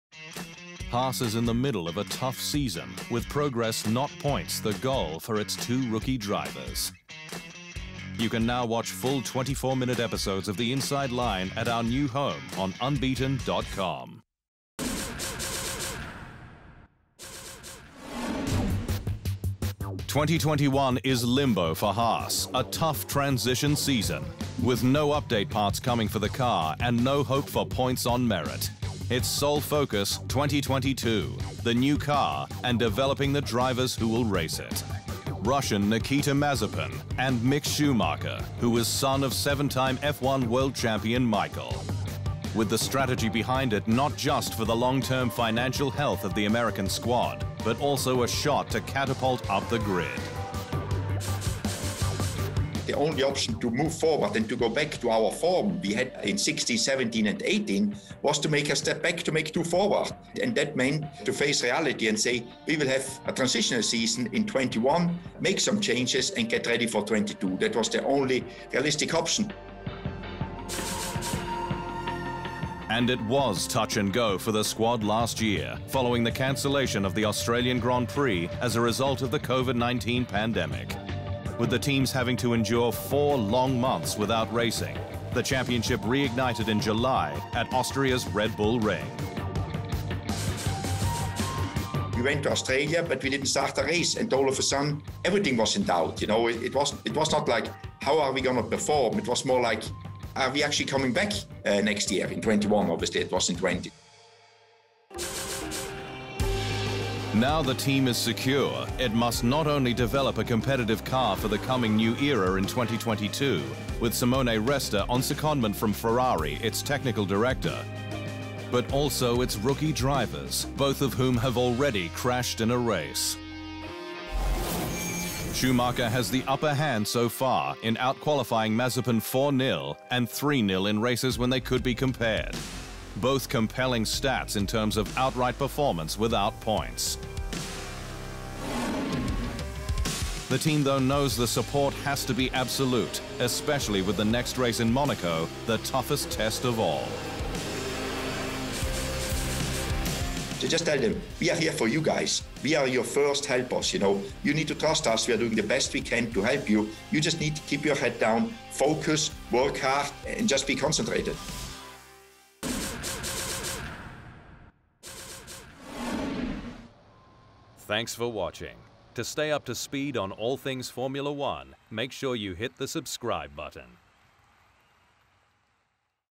Will Haas' transition season pay off in 2022?
2021 is limbo for Haas - a tough, transition season - with no update parts coming for the car, and no hope for points on merit. Its sole focus, 2022, the new car and developing the drivers who will race it.

That is: Russian Nikita Mazepin and Mick Schumacher, who is son of seven-time F1 World Champion Michael - with the strategy behind it not just for the long-term financial health of the American squad, but also a shot to catapult up the grid.

Follow @unbeatenf1 on Facebook, Twitter and Instagram.